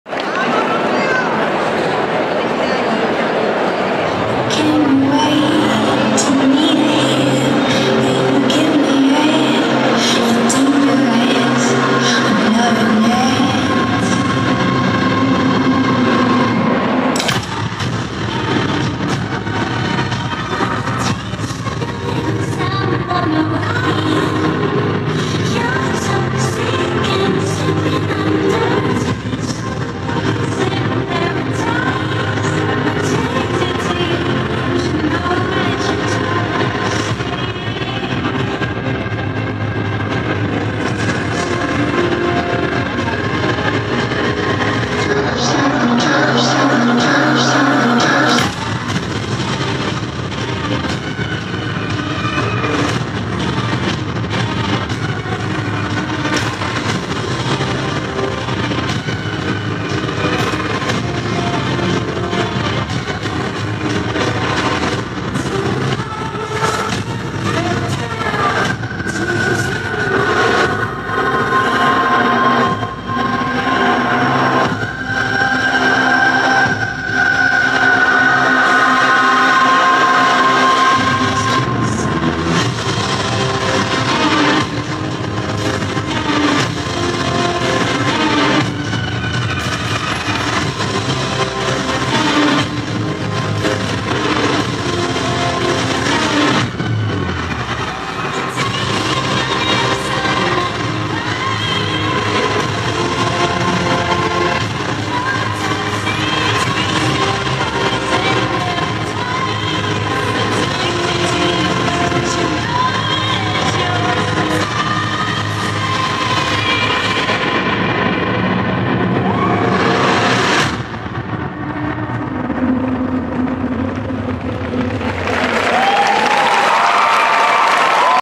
Japan Weekend Madrid (FEB 2024) - Concurso cosplay - Full Metal Alchemist
Concurso de cosplay en Japan Weekend Madrid (FEB 2024)
Actuación: Full Metal Alchemist